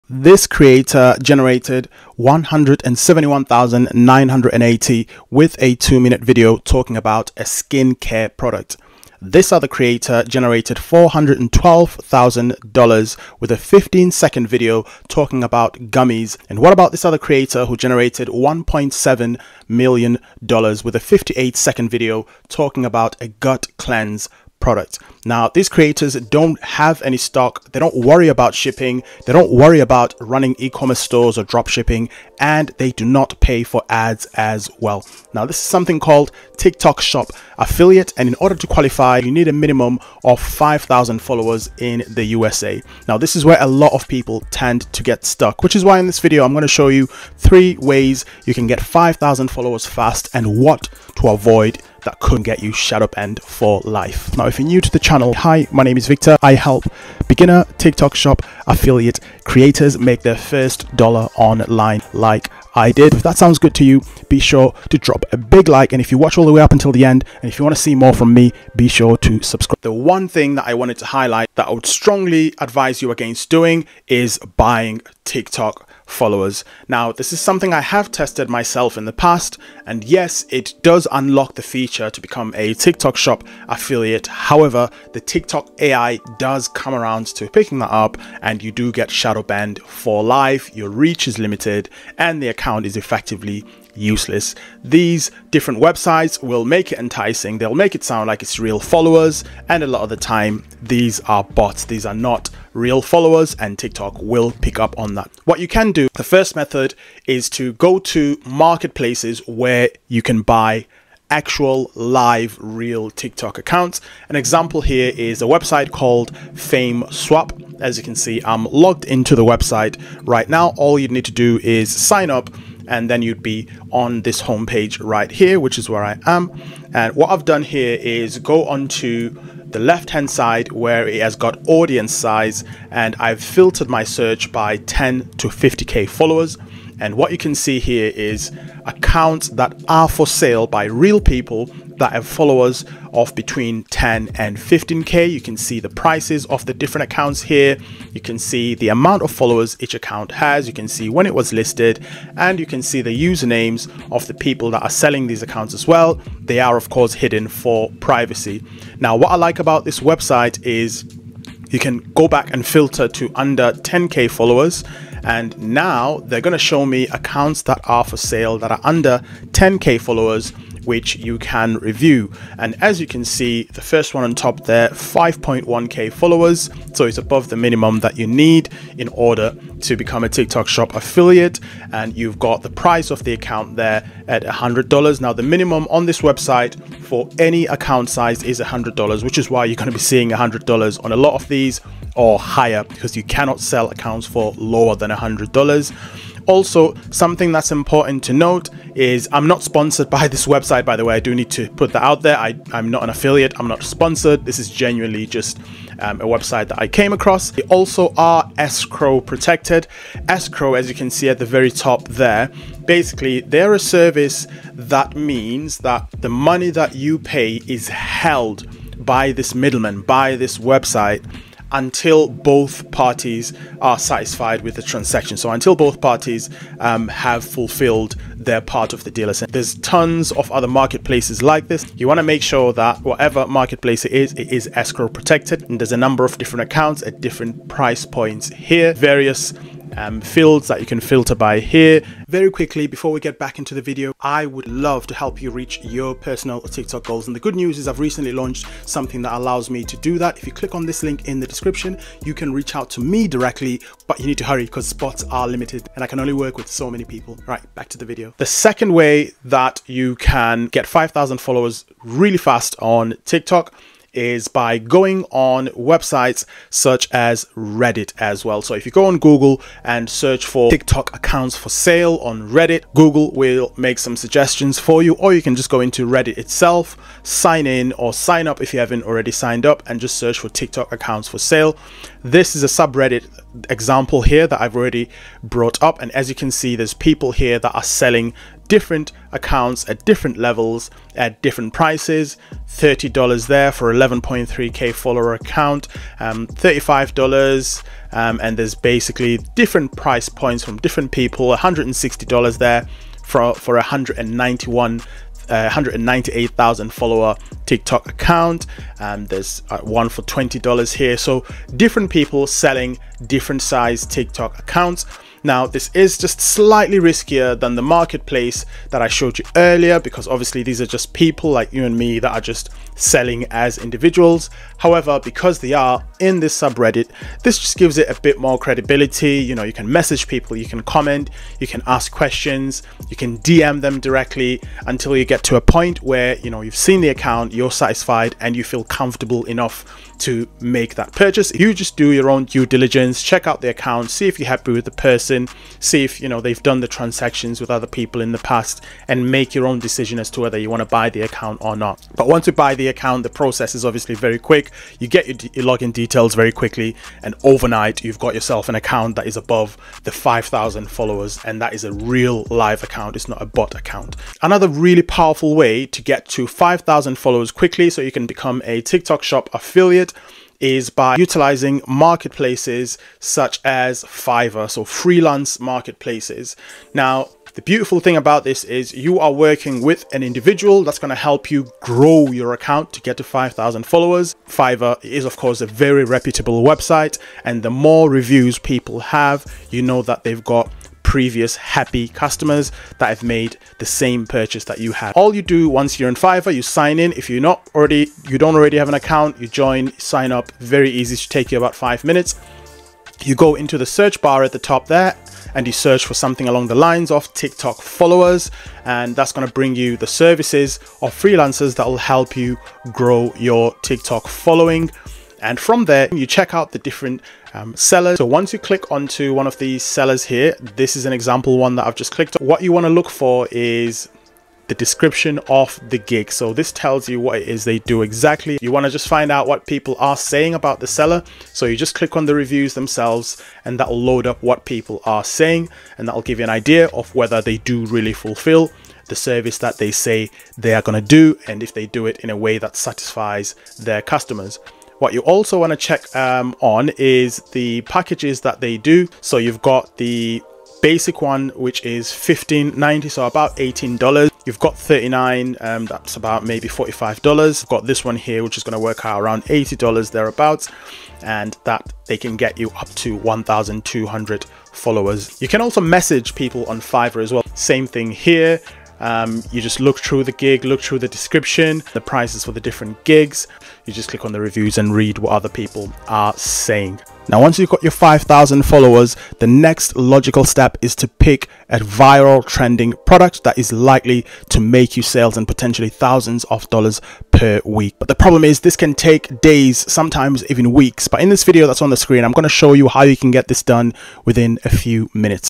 How To Get 5000 Tiktok Followers FAST [Tiktok Shop Affiliate Tutorial]
🥷 STEAL THESE FREE RESOURCES I CREATED JUST FOR YOU

🕵️ MY TIKTOK SHOP SPY TOOL (Use code: VICTOR for 15% off)

🕛 Timestamps
00:00 Intro
01:09 Avoid doing this
01:46 Method 1
04:18 Method 2
06:04 Method 3